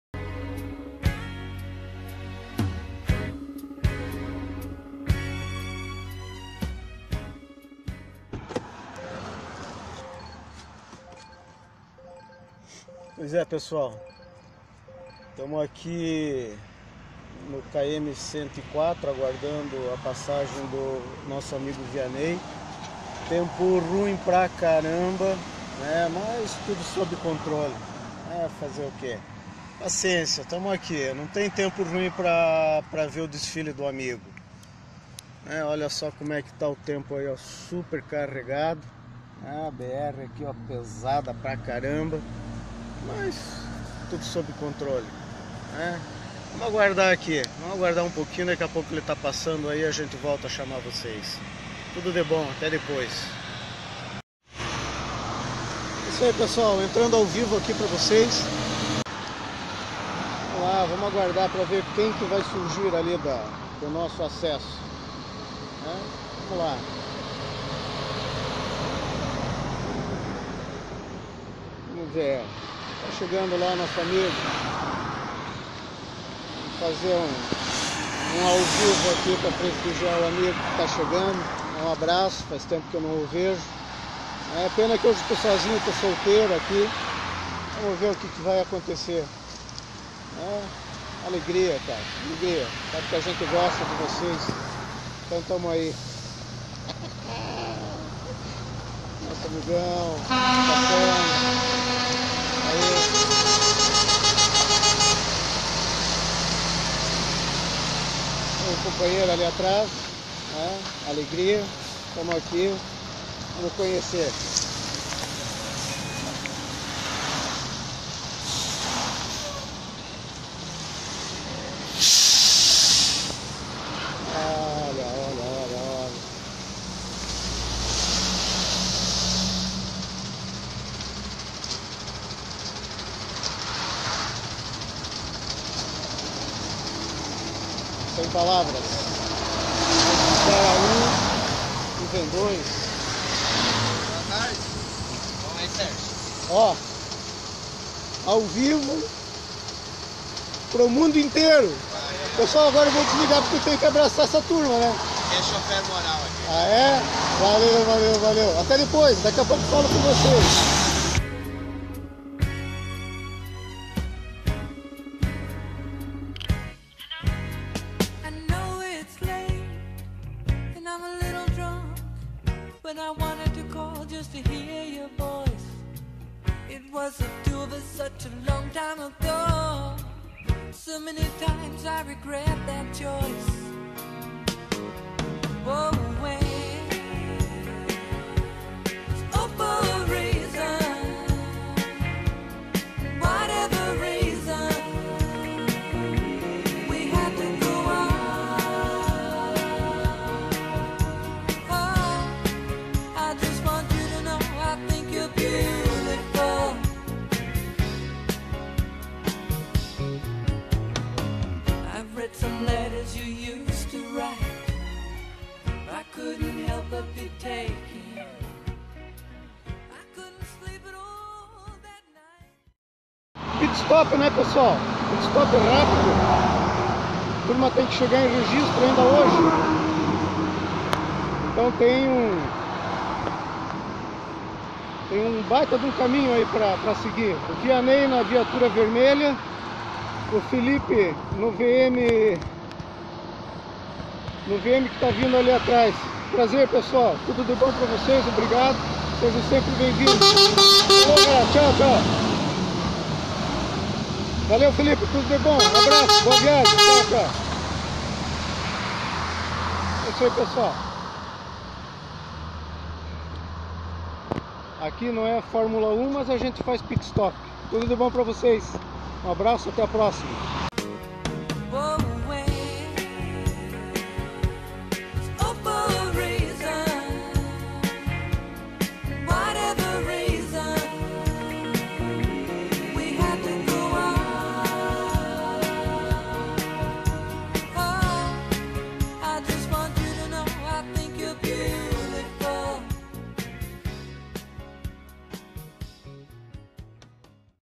Domingão do km 104 da Br 101 com Vianei e Felipe - Penha (SC)
Visita para munhecada rápida aqui no km 104 da Br 101 em Penha (SC). Agradecendo aos artistas do desfile pelo pit stop...